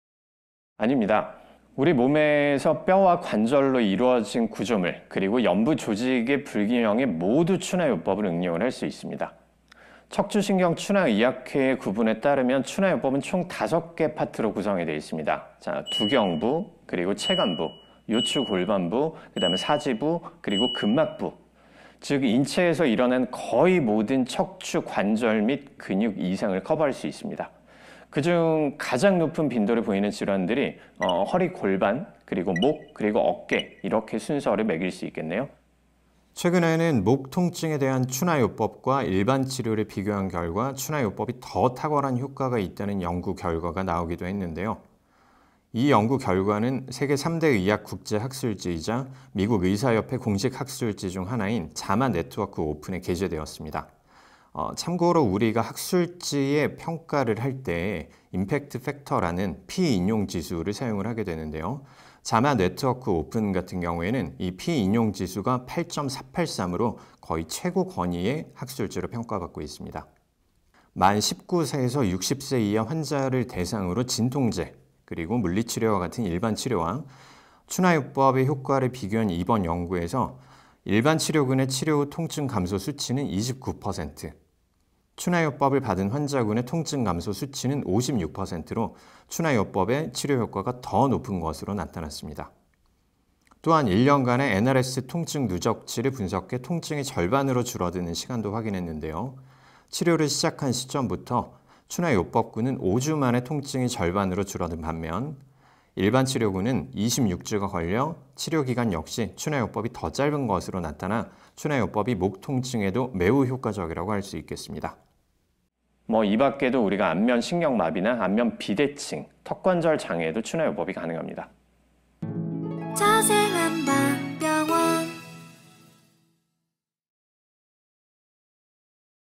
추나요법은 우리 몸 어느 부위에 받을 수 있는지 궁금하시죠?
자치모 ep06

안녕하세요! 여러분의 척추동반자 자생한방병원 유튜브입니다.
추나요법이 우리 몸 어느 부위에 받는 것인지,
정말 효과가 검증된 치료법인지 영상을 통해 확인해보세요!

척추관절 질환은 전문의의 진단과 처방이 가장 중요한 만큼,
증상이 있다면 내원해서 진단 받는것을 권유 드립니다.